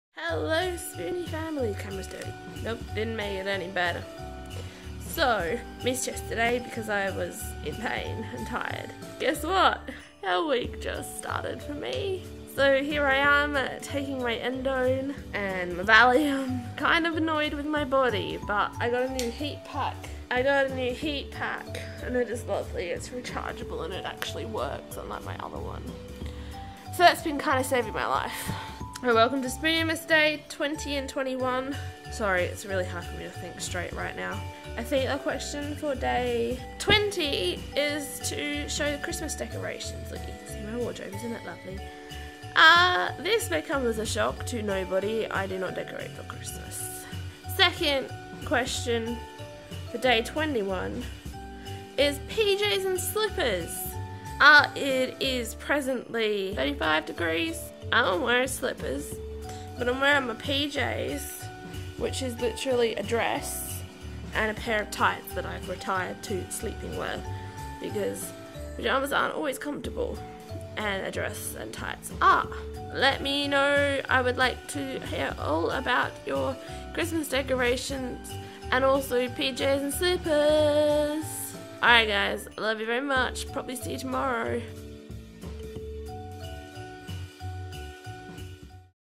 spooniemas Day 20 & 21: I'm a Scrooge... with No Pyjamas
CC Available: Happy Spooniemas Day 20 & 21!

Welcome to my own personal Hell Week / Shark Week / Flaremageddon. Yep.

Excited to chat to you in the comments and hear your answers all month :-)

Chronicles Theme Music by
+ Images by Jazz Acuña | @jazzacunaimages (Facebook)